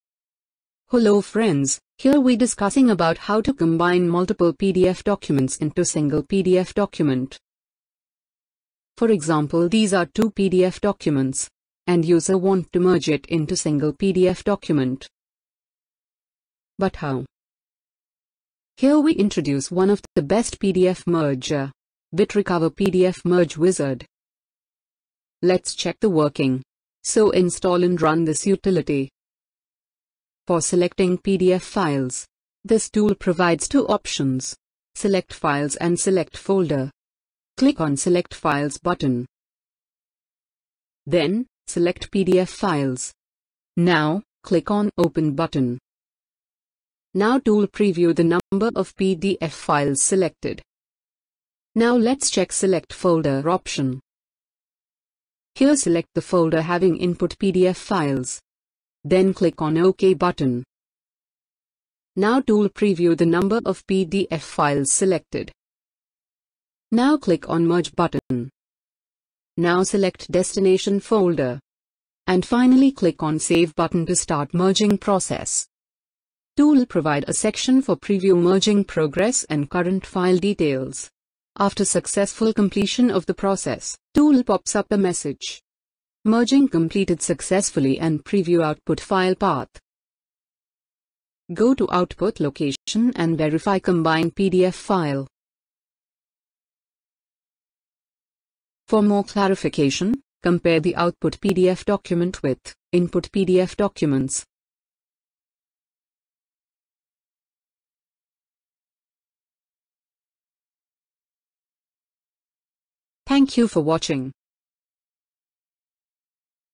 How to Combine PDF files without Acrobat - Unite PDF files
Learn, How to combine PDF files into one single PDF document. Unite PDF files into one.